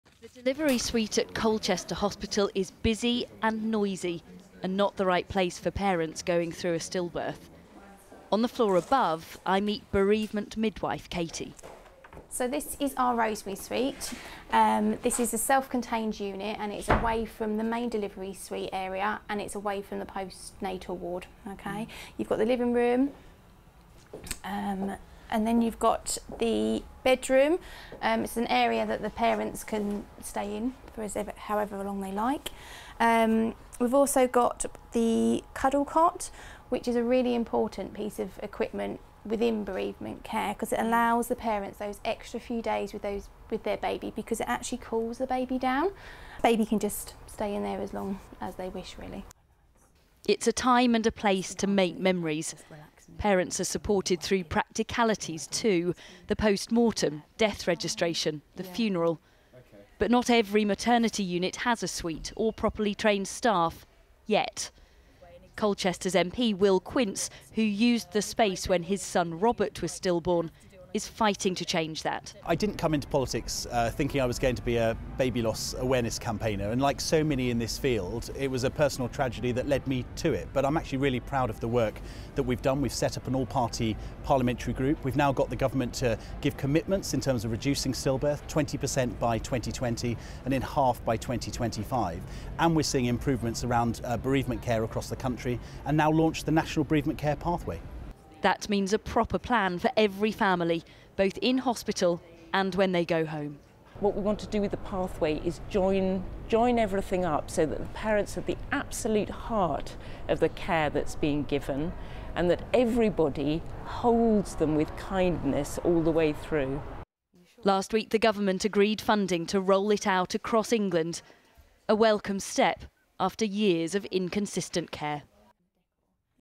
What does a stillborn baby bereavement suite look like? - 5 News
We're shown around Colchester hospital to see what is offered to parents shortly after a stillbirth. Will Quince MP and Dr Clea Harmer explain why more needs to be done to help grieving families.